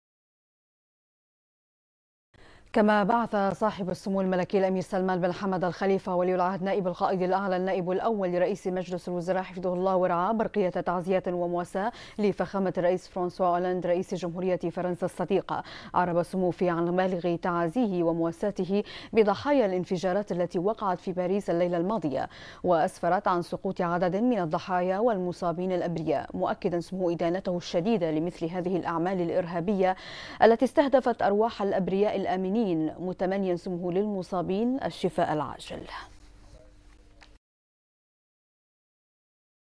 البحرين: سمو ولي العهد يرسل برقية تعزية إلى رئيس جمهورية فرنسا
المنامة في 14 نوفمبر/ بنا / بعث صاحب السمو الملكي الامير سلمان بن حمد ال خليفة ولي العهد نائب القائد الاعلى النائب الاول لرئيس مجلس الوزراء حفظه الله ورعاه برقية تعزية ومواساة لفخامة الرئيس  فرانسوا أولاند رئيس جمهورية  فرنسا الصديقة ،اعرب سموه فيها عن بالغ تعازيه ومواساته بضحايا الانفجارات التي وقعت في باريس الليلة الماضية، وأسفرت عن سقوط عدد من الضحايا والمصابين الابرياء.